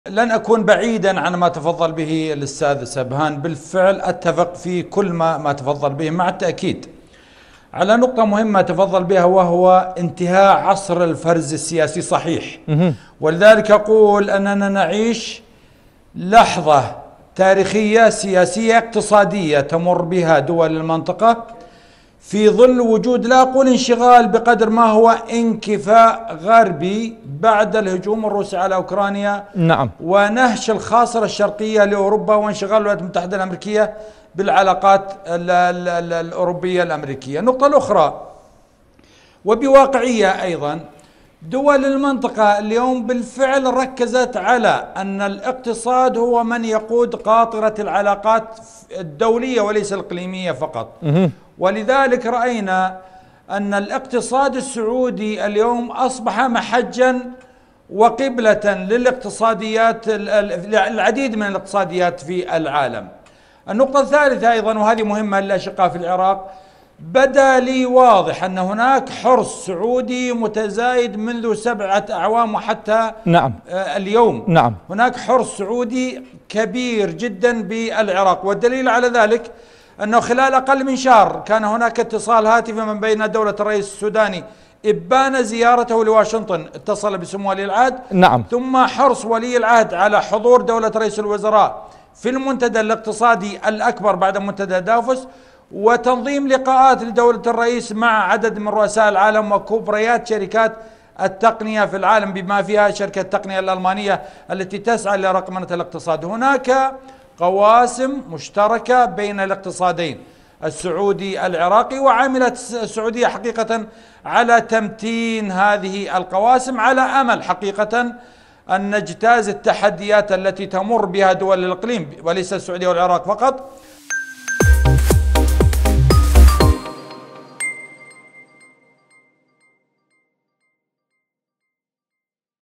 مبارك العاتي: هناك حرص سعودي على تمتين العلاقات الثنائية مع العراق
السوداني في السعودية.. تأكيد على الشراكة والارتقاء بالعلاقة واجب_الصراحة مع علي عماد
الضيوف:
سبهان الملا جياد | المستشار السياسي لرئيس مجلس الوزراء
د. عبد الرحمن المشهداني | خبير اقتصادي
مبارك العاتي | محلل سياسي